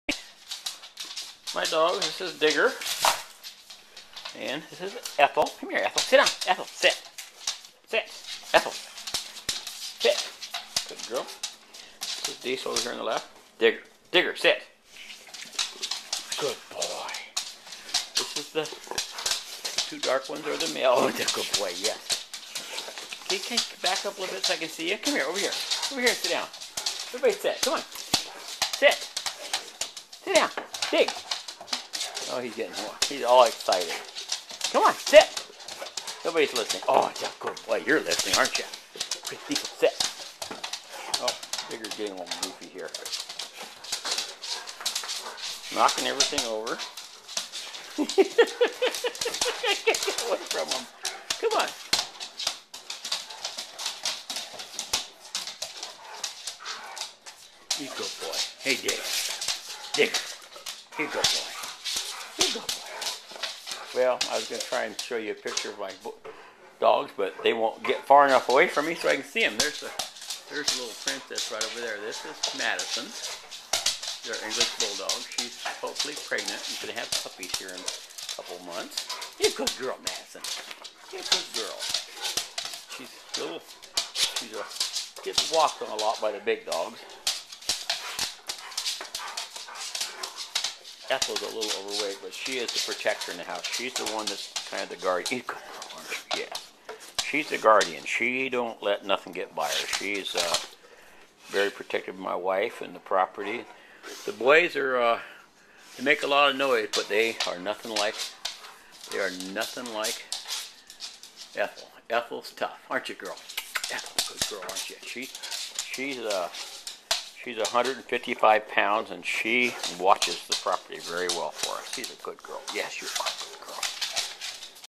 OUR THREE BULLMASTIFFS AND ONE BULLDOG
TRYING OUT MY NEW FLIP CAMERA . WSP TOOK MY FIRST CAMERA   AS EVIDENCE IN THE TROOPER SHOOTING . MY WIFE PICKED THIS ONE UP FOR ME TODAY .    IT'S A LITTLE DIFFERNT MODEL AND I WANT TO SEE  WHAT QUAILITY VIDEO  IT GIVES